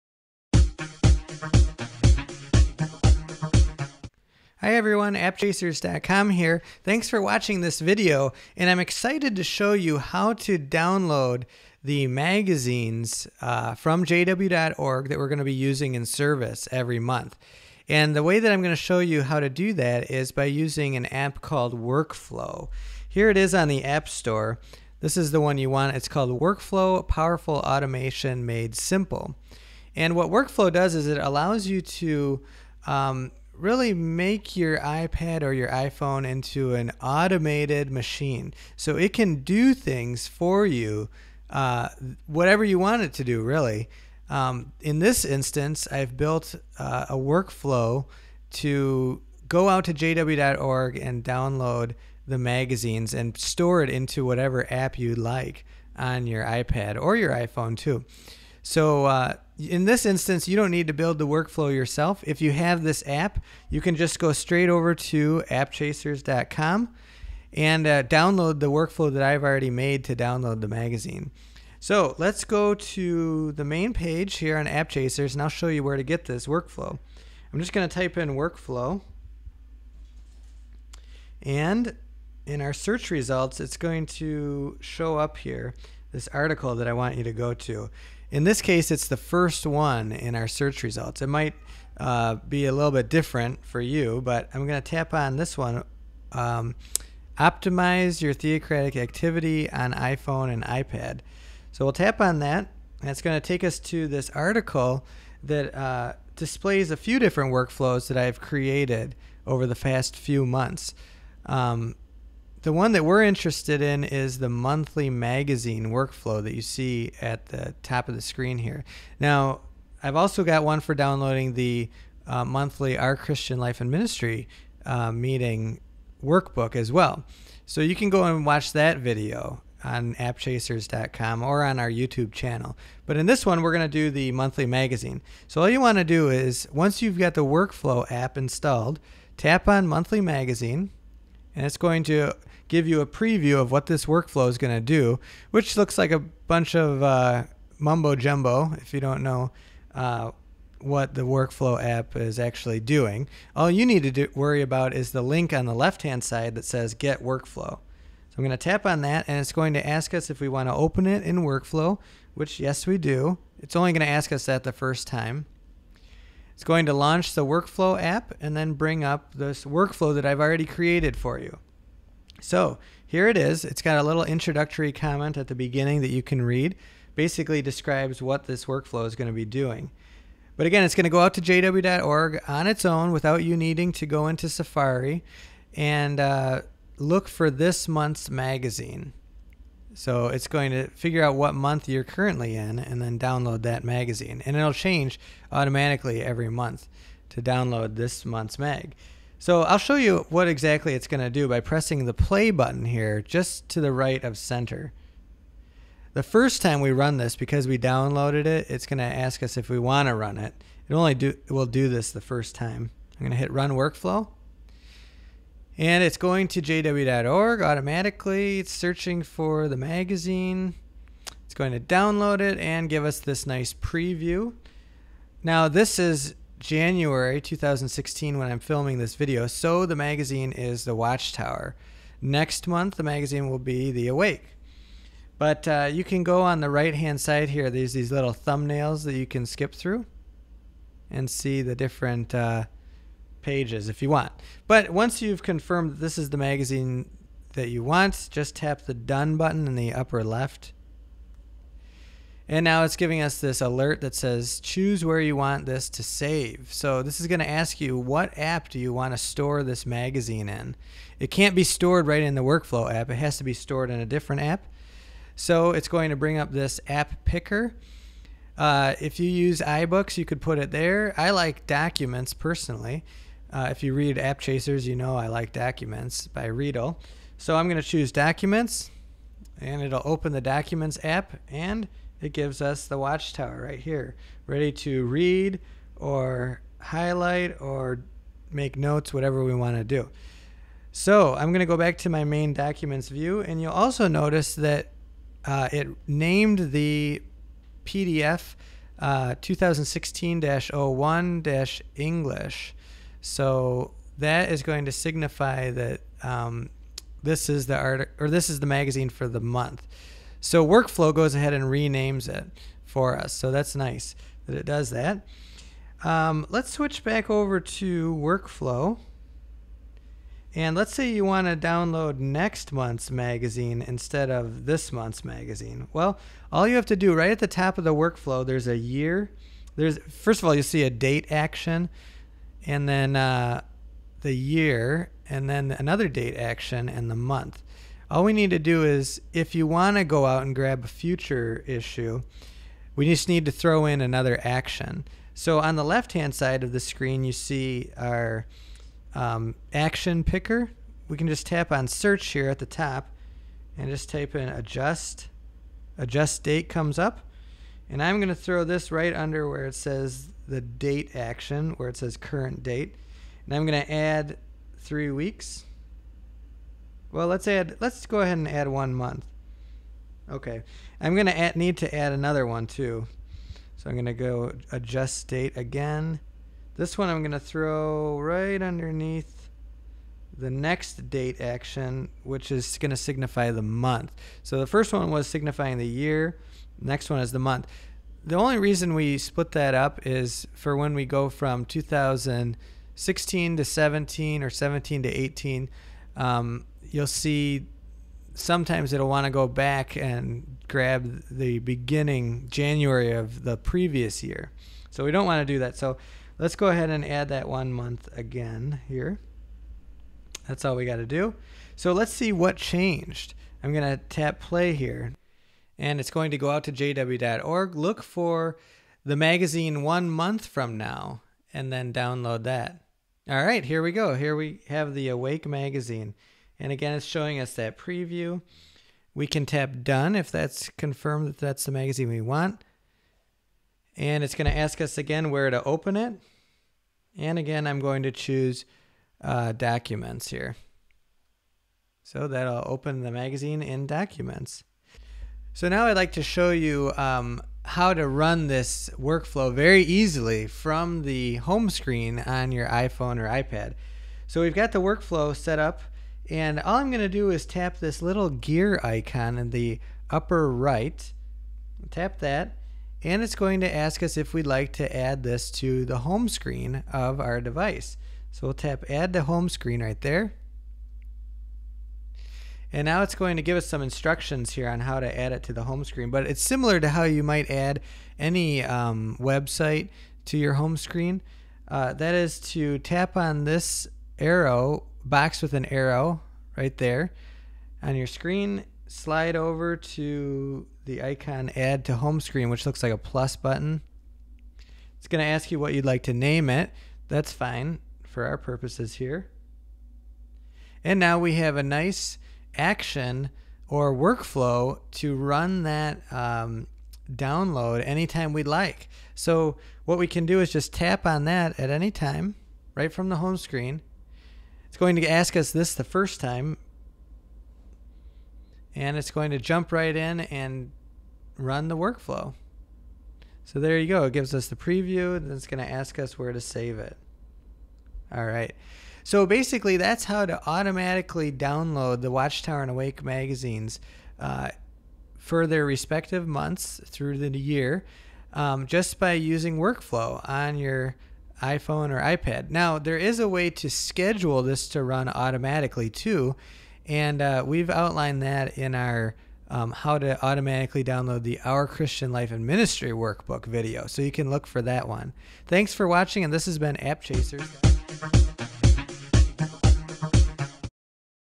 Automatically download Watchtower and Awake Magazines to your iPhone or iPad
In this video my end goal is to automatically download the Watchtower and Awake magazines every month without having to remember to do it. So I built a workflow to do exactly that.It will go out to JW.org, find the current month's workbook, download it, show you a preview, and then allow you to open it in iBooks or another app you'd like to use (I prefer Documents). You don't have to build this workflow yourself, you can just grab the one I've already made at the following link.